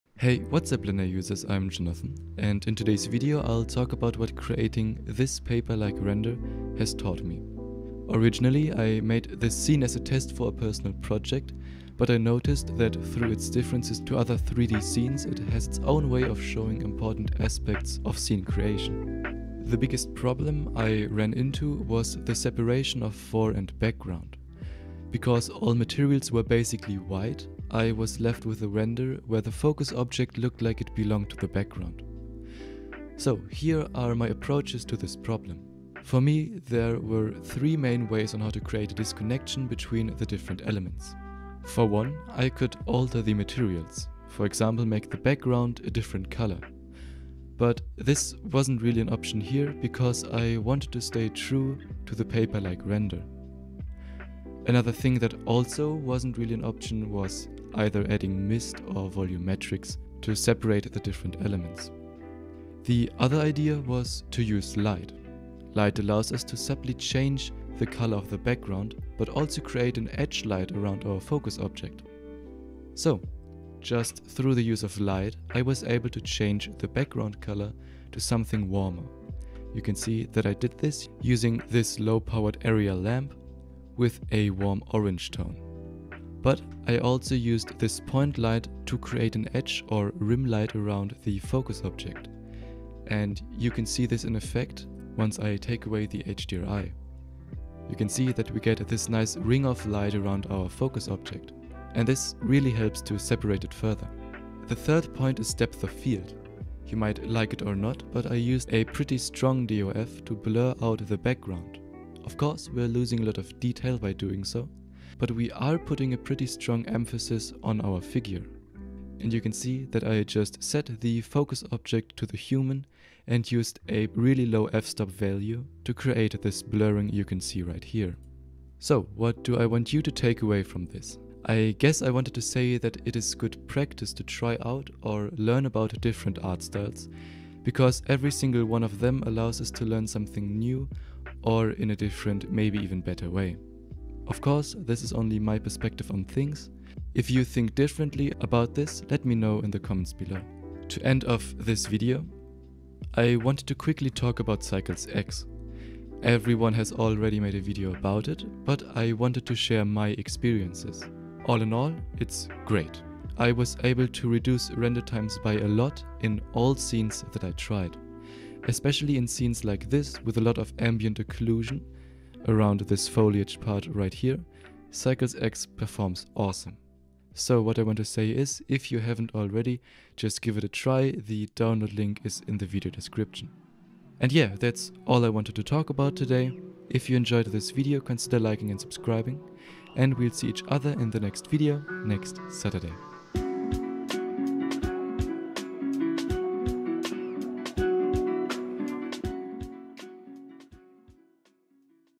Paper Render in Blender [and its lessons] | English
In this tutorial, I show you how to create a paper-like render and what to "lookout for".

**Partners**